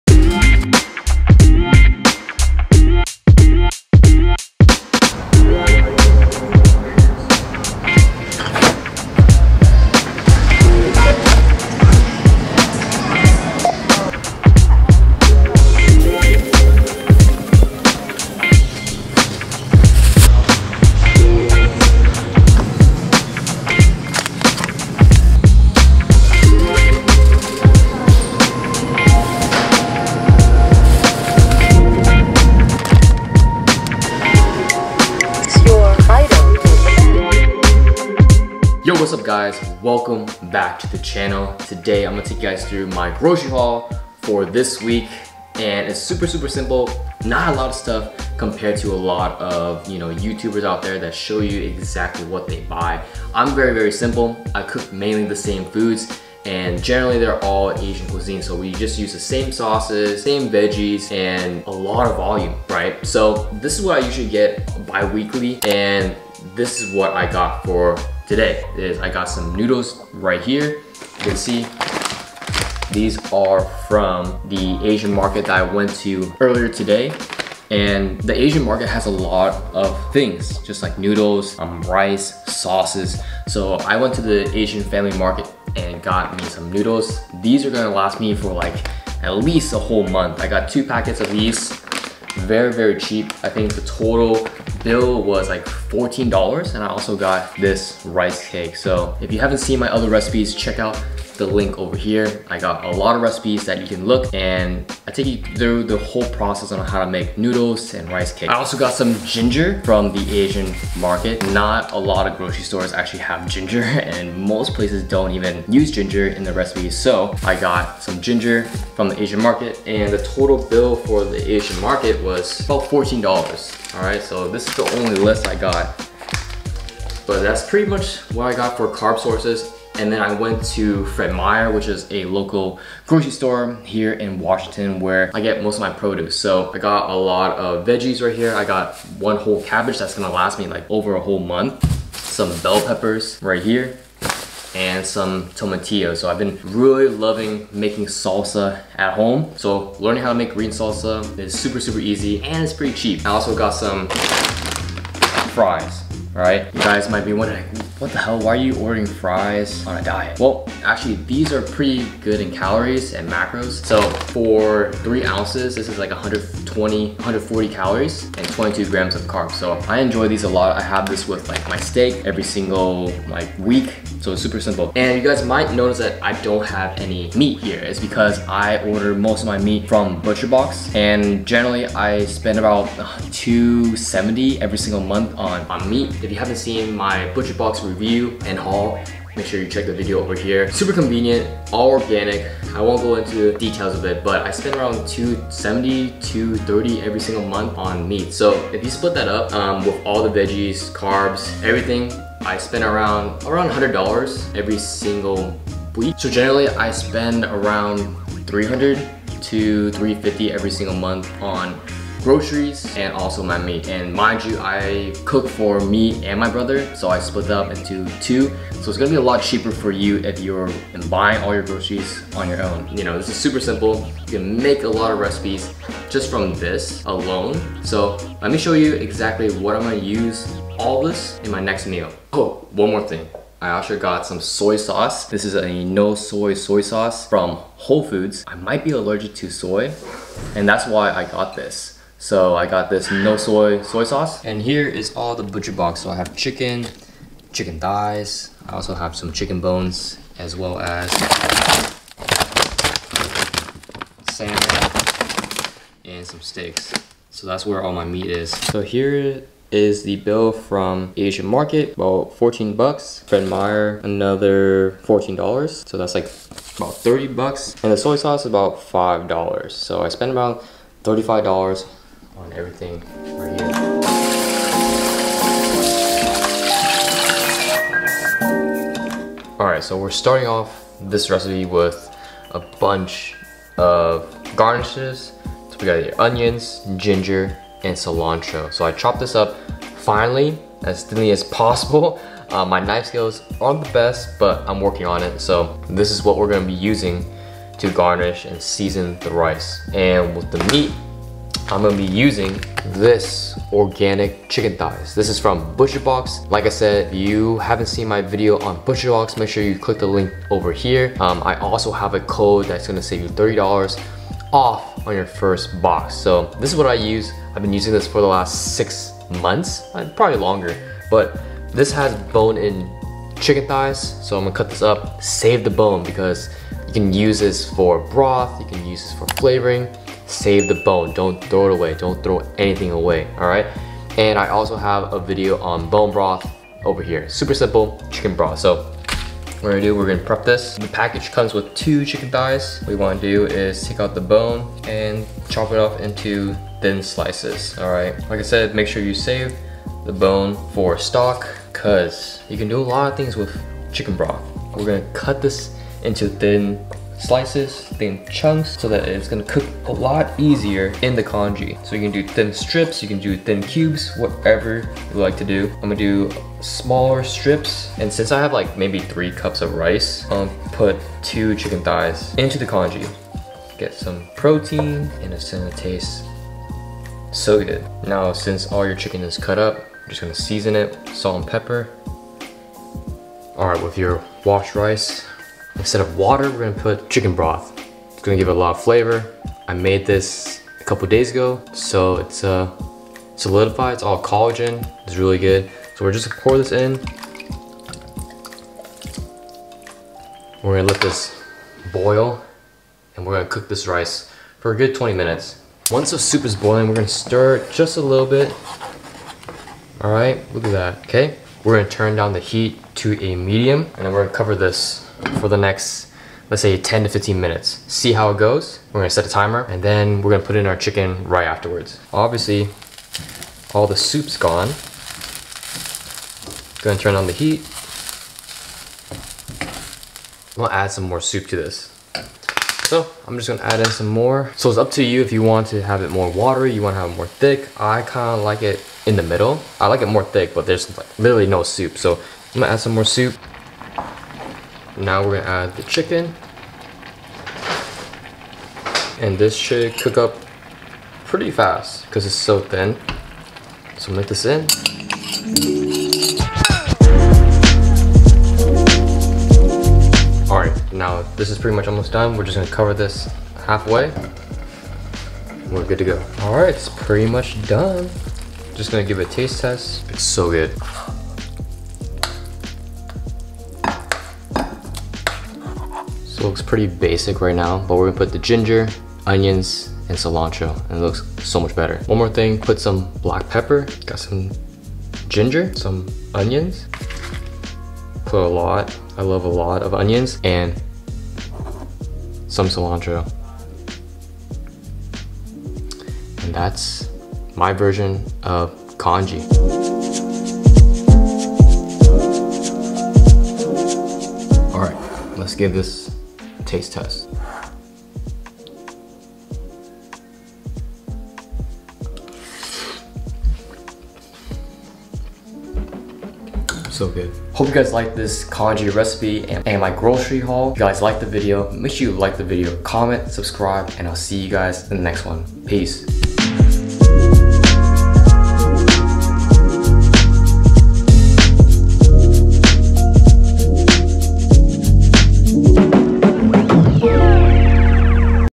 What I Buy On a Budget to Lose Fat | Chicken Congee Recipe for the Fall Season!
Get Lean Eating Asian Cuisine: asianbossboy.com

00:00 Grocery Haul
04:33 Chicken Congee Recipe
05:00 Butcher Box Discount
05:39 Cooking Congee
08:24 Taste Test

About:
In this video, I do my normal grocery haul at the Asian Family Market, Fred Meyer, and more!

As you can see from the video, my budget is around $50 for 2-3 weeks and all my protein sources are from Butcher Box.

Overall, I spend about $300-350 on groceries a month!

You can definitely spend less by shopping for discounted meats at Costco and at your local grocery store.

I also used some ingredients to make an easy chicken congee recipe because it's perfect for the fall season and it has a lot of protein.

Hope you guys enjoy this video and let me know if it was helpful!